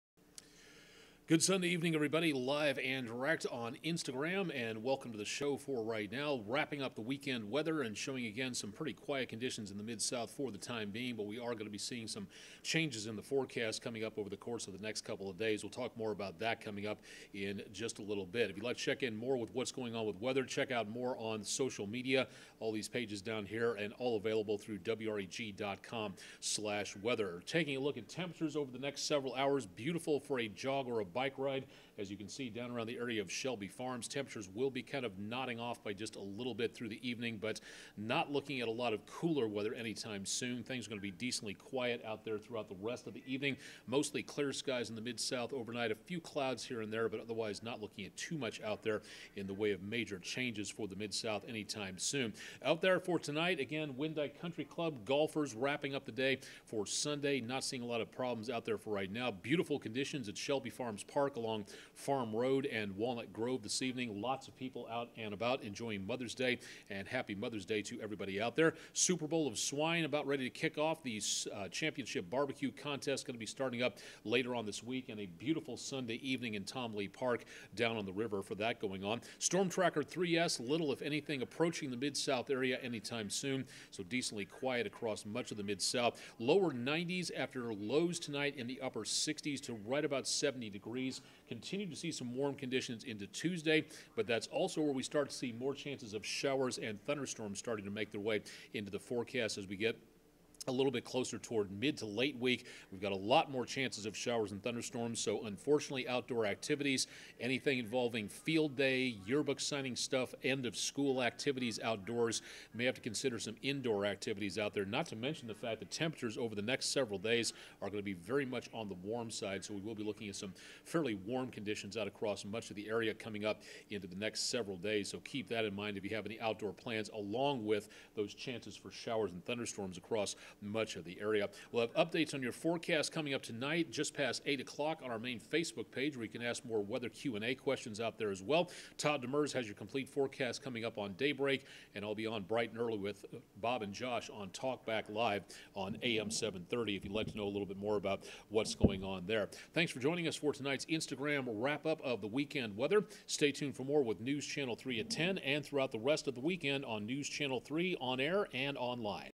WREG-TV Instagram Weather for Sunday PM 5.13.18
WREG Meteorologist Austen Onek takes a quick look at the end of the weekend weather on the Instagram account.